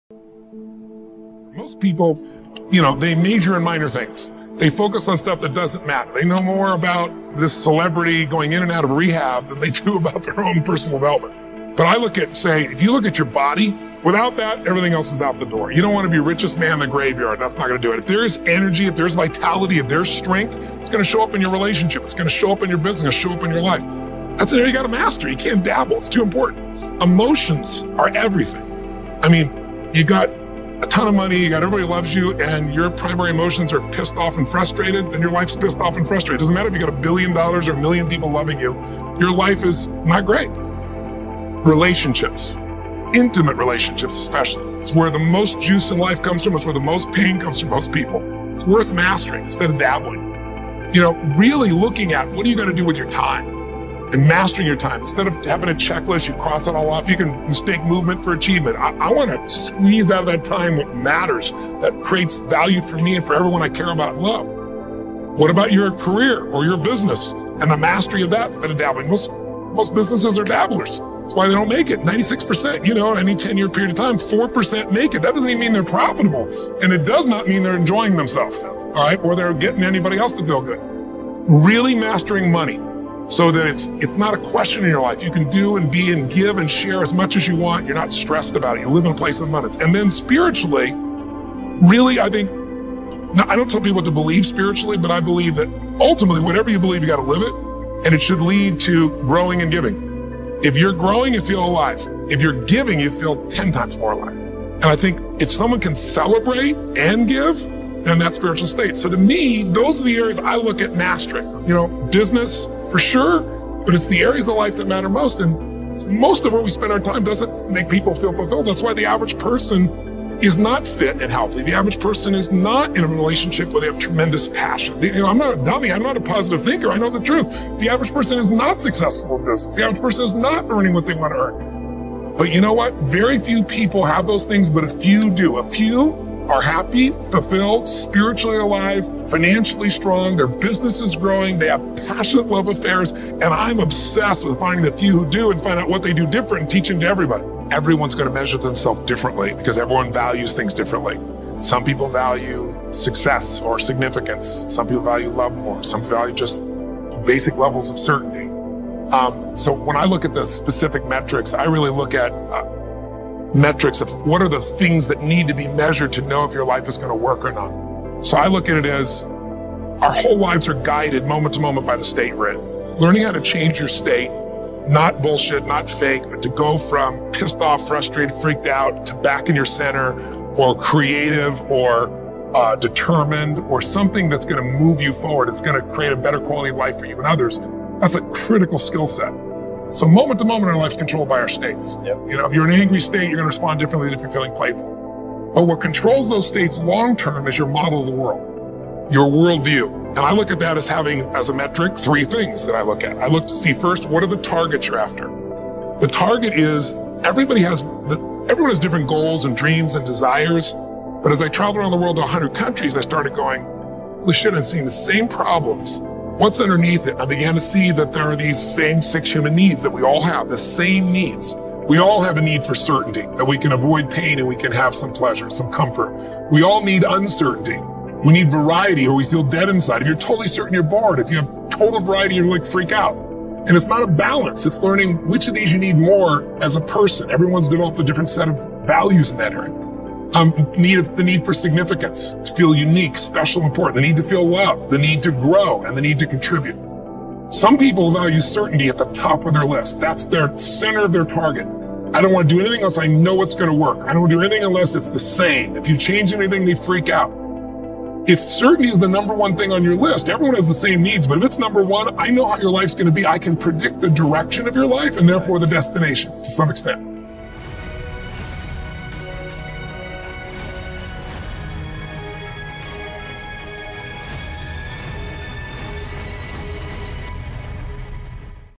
Refresh Your Mindset Motivational Speech 2022
"Learn How to Think Correctly and You Will Achieve Anything" - The Mindset of a Billionaire
0:01 – Into
0:22 - That's it you got to master
1:37 - growing and giving
3:22 - what are the targets you're after
5:05 - More anything is not going to give you more
Speaker: - Tony Robbins